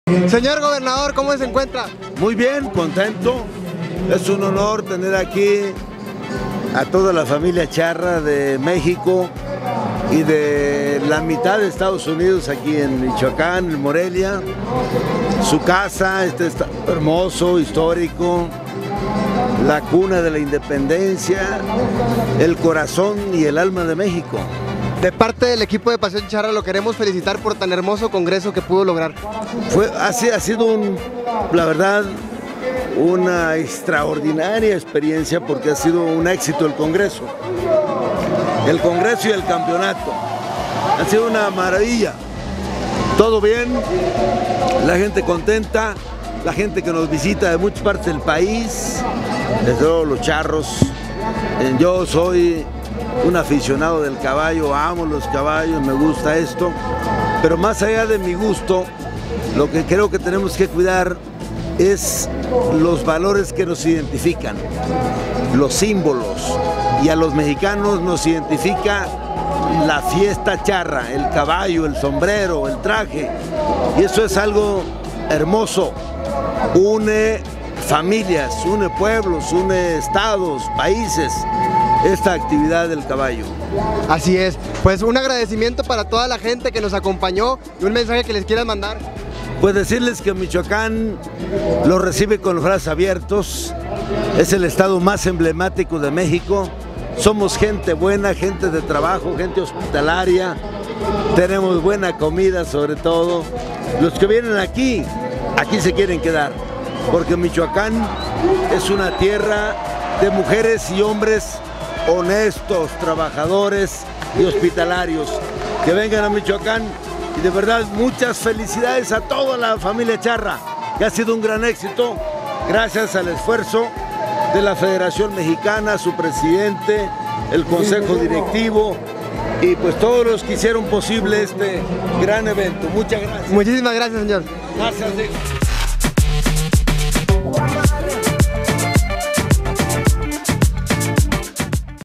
Ing. Silvano Aureoles Conejo Gobernador de Michoacán
Nos acompaña el Ing. Silvano Aureoles Conejo, Gobernador del Estado de Michoacán para platicar de este gran cierre del Congreso y Campeonato Nacional Charro Michoacán 2019